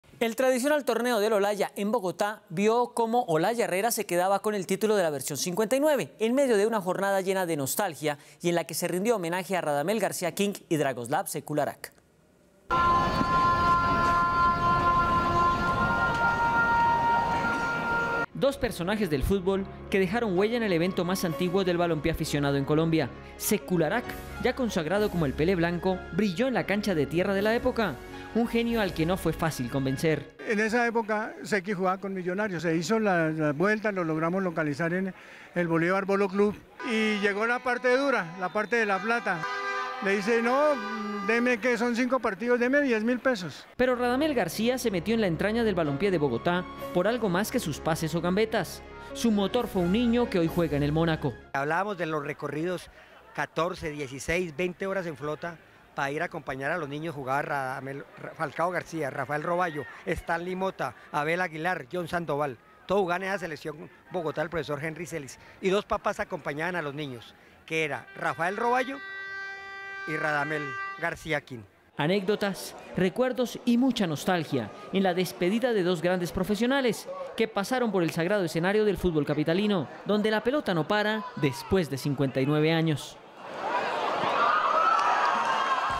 RED+ | Homenaje a la memoria de Radamel García y Sekularac en el hexagonal del Olaya
El tradicional torneo del Olaya en Bogotá vió hoy como Olaya Herrera se quedaba con el titulo de la versión 59 ... en medio de una jornada llena de nostalgía y en la que se rindió homenaje a Radamel García King y Dragoslav Sekularac